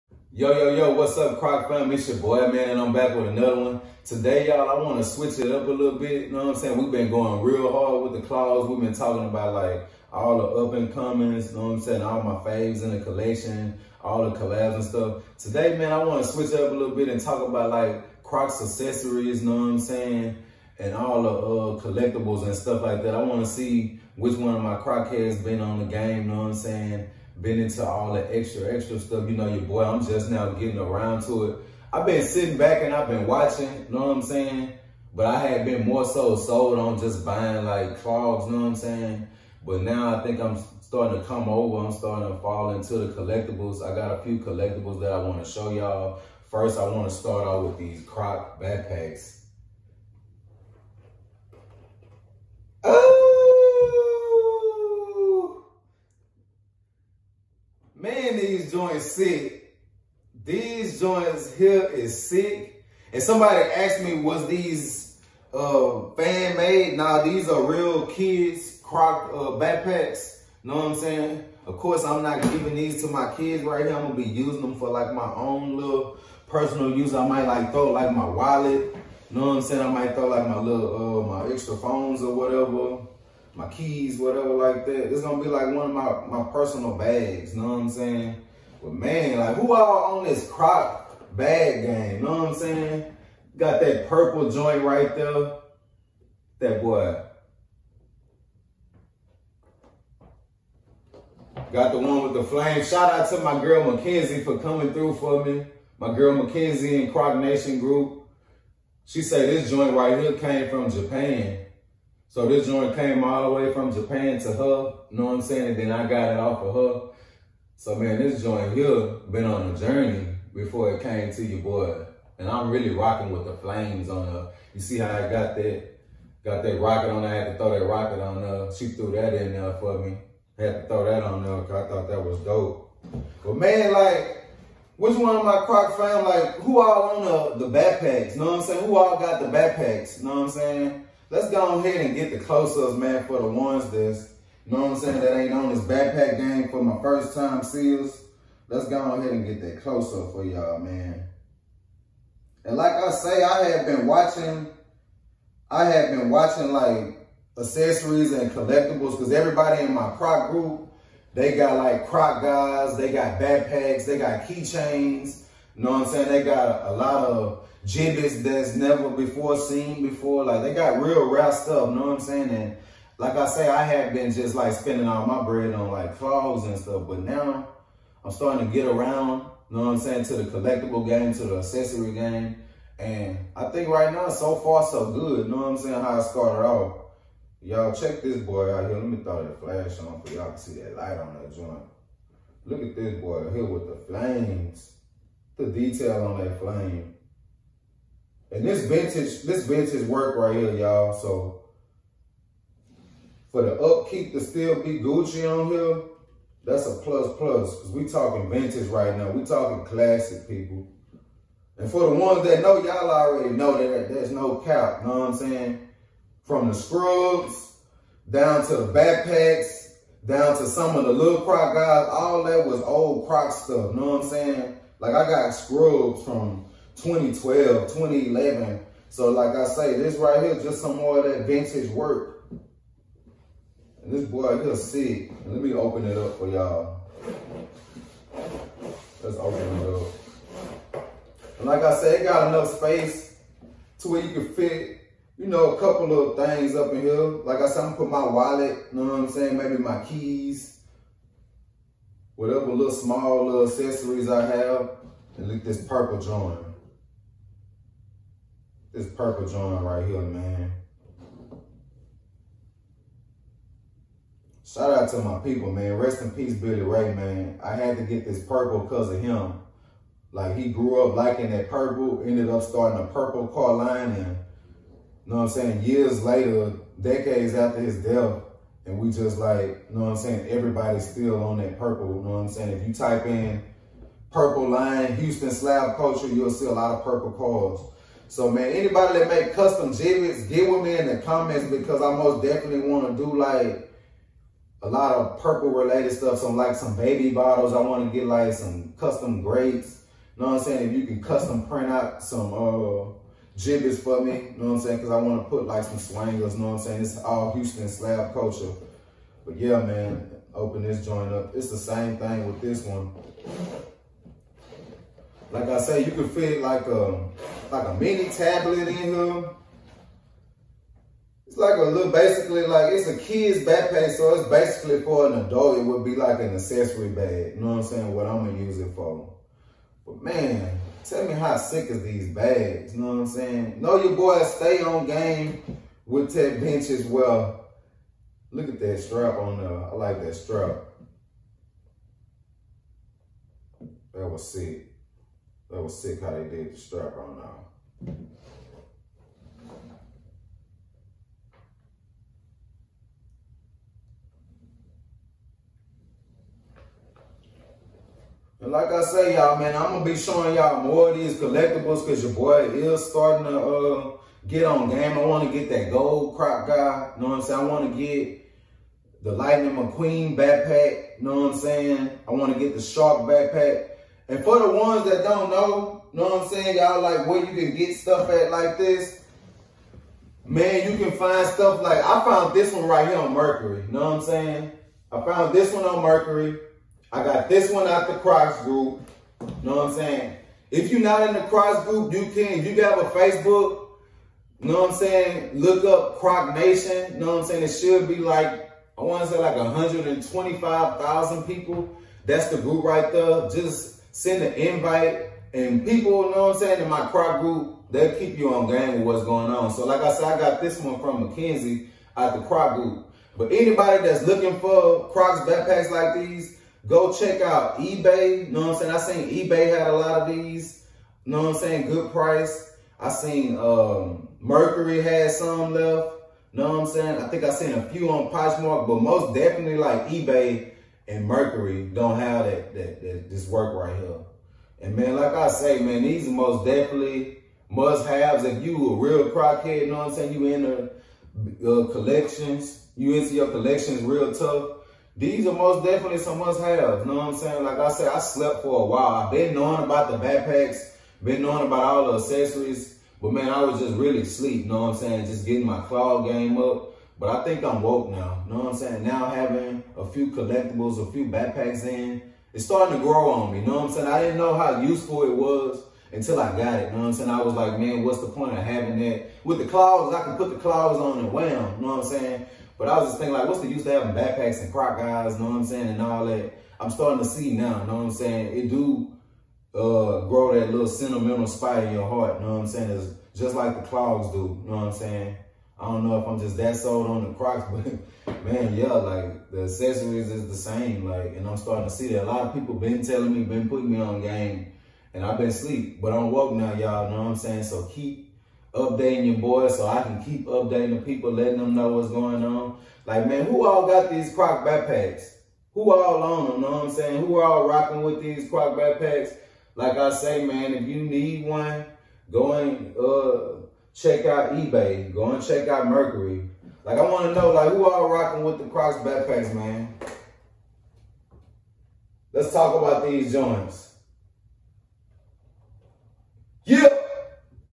Kids Backpack x Crocs Review
Using crocs kids backpack as my lil extra’s bag to keep things out my pocket

Reviews Ratings Sizing

Anytime you shop at crocs use yo boy link help me build my rep ❤️🤞🏾